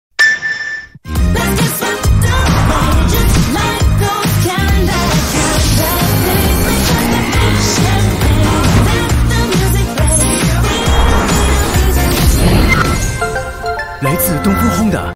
Dongfeng Honda Lingxi L 2025
Promotional video of new EV of Dongfeng-Honda Lingxi L

Source: Dongfeng-Honda